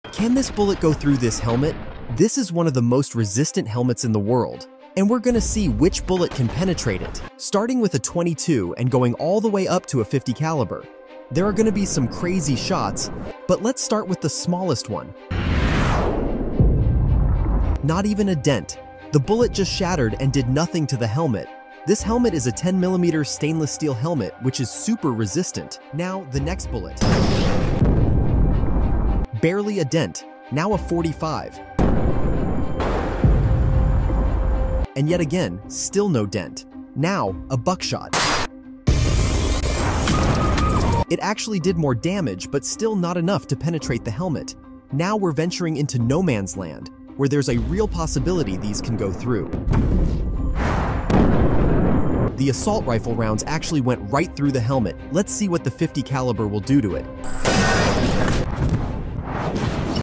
Can this BULLET go through this HELMET?🪖
Every Gun VS Bulletproof Master Chief Helmet!
Can this 10mm stainless steel helmet stand up to some of the most powerful bullets in the world? We put it to the ultimate test, starting with a small .22 caliber and working our way up to a .50 caliber sniper round. Will it hold up, or will it be obliterated?

This video was inspired by "Impact Props" Go follow their channel for more details video and crazy content like this.

💥 Bullet Tests:
- .22 Cal: No dent, bullet shattered.
- 9mm: Barely a scratch.
- .45 Cal: No damage at all.
- Buckshot: More damage, but still no penetration.
- Assault Rifle Rounds: Went right through the helmet.
- .50 Cal: Total destruction—helmet obliterated!